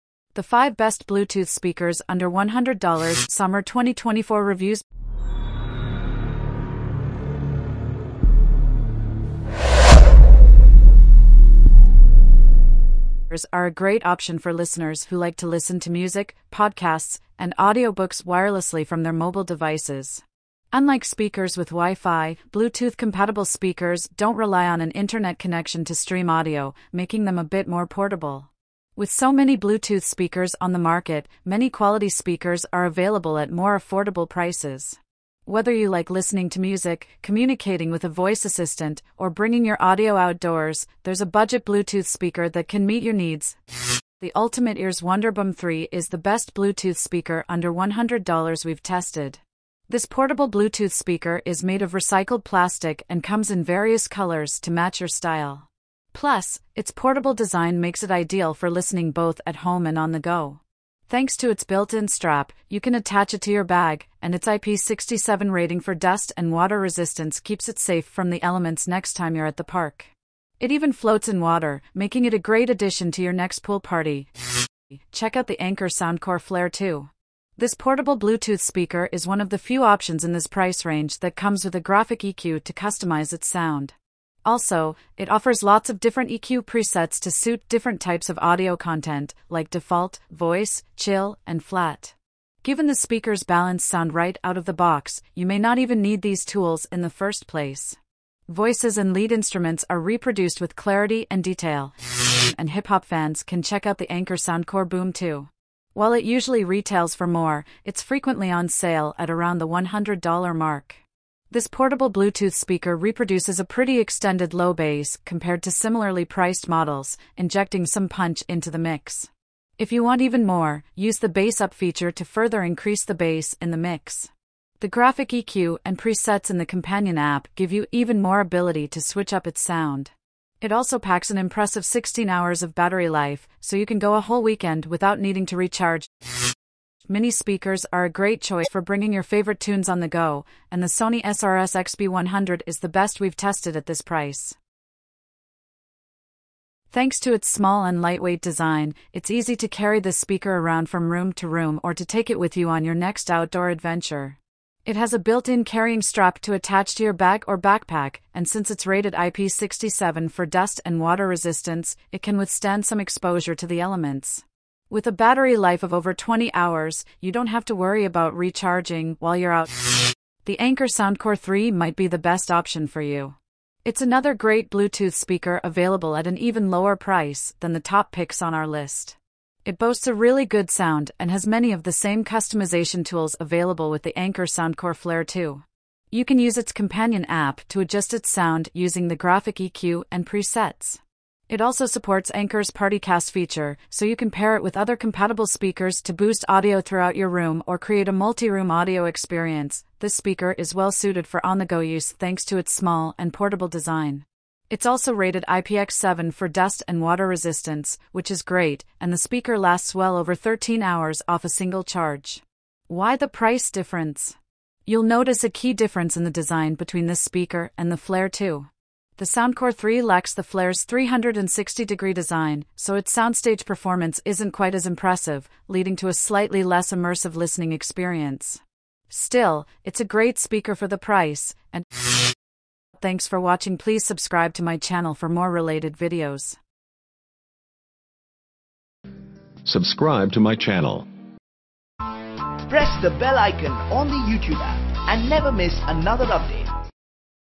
Best 5 Bluetooth Speakers under $100 dollars / 2024 Bluetooth speakers Reviews
Bluetooth speakers are a great option for listeners who like to listen to music, podcasts, and audiobooks wirelessly from their mobile devices. Unlike speakers with Wi-Fi, Bluetooth-compatible speakers don't rely on an internet connection to stream audio, making them a bit more portable. With so many Bluetooth speakers on the market, many quality speakers are available at more affordable prices. Whether you like listening to music, communicating with a voice assistant, or bringing your audio outdoors, there's a budget Bluetooth speaker that can meet your needs.